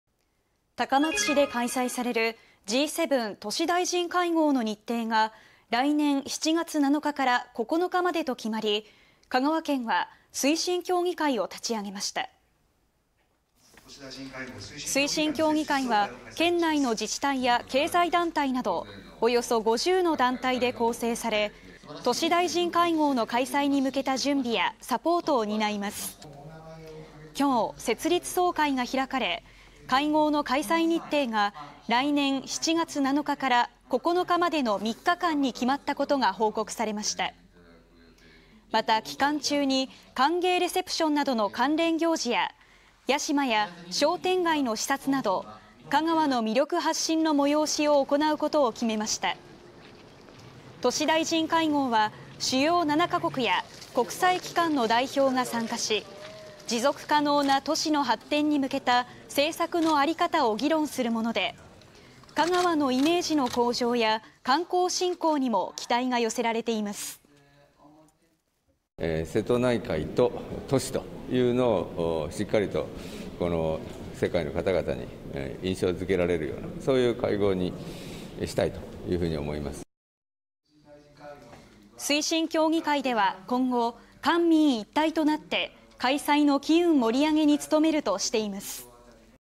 「Ｇ７都市大臣会合」２３年７月開催 官民一体となって開催の機運盛り上げ【香川・高松市】 (22/11/01 18:00)
高松市で開催されるＧ７都市大臣会合の日程が、２０２３年７月７日から９日までと決まり、香川県は推進協議会を立ち上げました。

推進協議会は、県内の自治体や経済団体など、約５０の団体で構成され、都市大臣会合の開催に向けた準備やサポートを担います。

１１月１日設立総会が開かれ、会合の開催日程が、２０２３年７月７日から９日までの３日間に決まったことが報告されました。

また、期間中に歓迎レセプションなどの関連行事や、屋島や商店街の視察など、香川の魅力発信の催しを行うことを決めました。

都市大臣会合は、主要７カ国や国際機関の代表が参加し、持続可能な都市の発展に向けた政策の在り方を議論するもので、香川のイメージの向上や観光振興にも期待が寄せられています。

（香川県　池田豊人知事）
「瀬戸内海と都市を世界の人に印象付けられるような会合にしたい」

推進協議会では、今後、官民一体となって開催の機運盛り上げに努めるとしています。